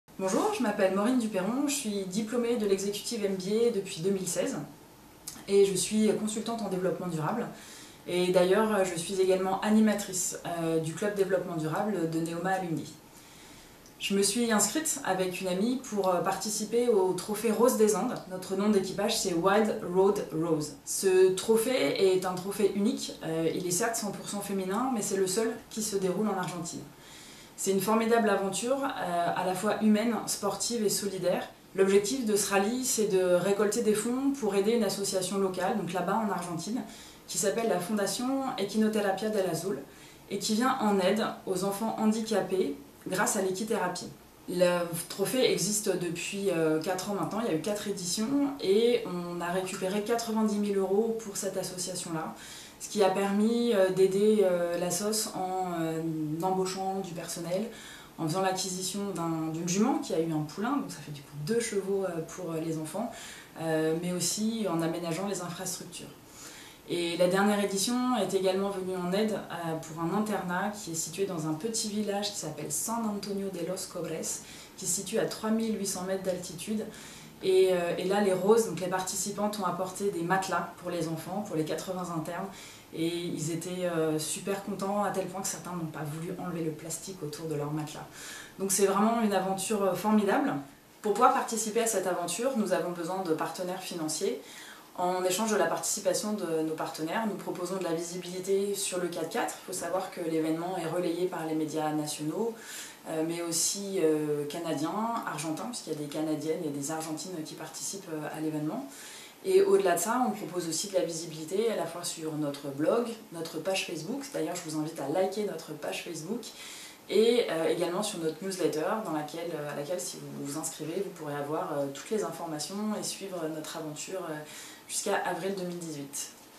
Maureen Dupeyron se lance dans le Trophée Roses des Andes
Maureen Dupeyron (Executive MBA 2016) se lance dans le Trophée Roses des Andes en Argentine.

NEOMA ALUMNI est l'association des diplômés de NEOMA Business School. Nous sommes partenaires des alumni de l'école et de leur rayonnement.